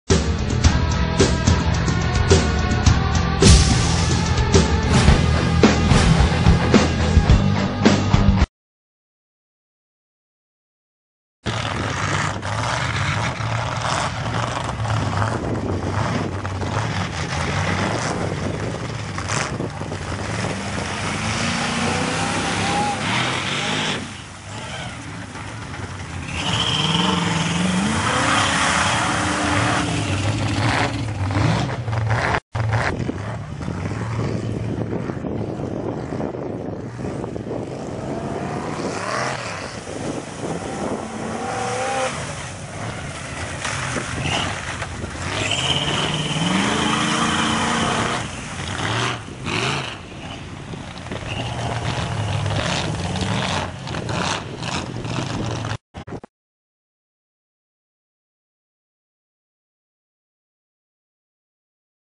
Old Blue Ford Mudding At Back 40 Mud Bog
Footage from the May 11 2013 Back 40 Boggers Mud Bog
Located in Michigan